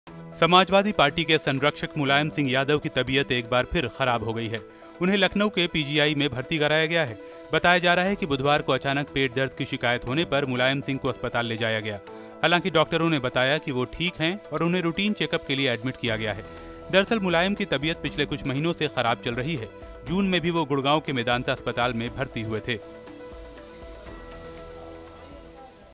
सपा संरक्षक मुलायम सिंह यादव की तबीयत फिर खराब, लखनऊ PGI में भर्ती
समाजवादी पार्टी के संरक्षक मुलायम सिंह यादव की तबीयत एक बार फिर खराब हो गई है. उन्हें लखनऊ के पीजीआई में भर्ती कराया गया है. बताया जा रहा है कि बुधवार को अचनाक पेट दर्द की शिकायत होने पर मुलायम सिंह को अस्पताल ले जाया गया. हालांकि डॉक्टरों ने बताया कि वो ठीक हैं और उन्हें रूटीन चेकअप के लिए एडमिट किया गया है. दरअसल मुलायम की तबीयत पिछले कुछ महीनों से खराब चल रही है, जून में भी वो गुंडगांव के मेदांता अस्पताल में भर्ती हुए थे.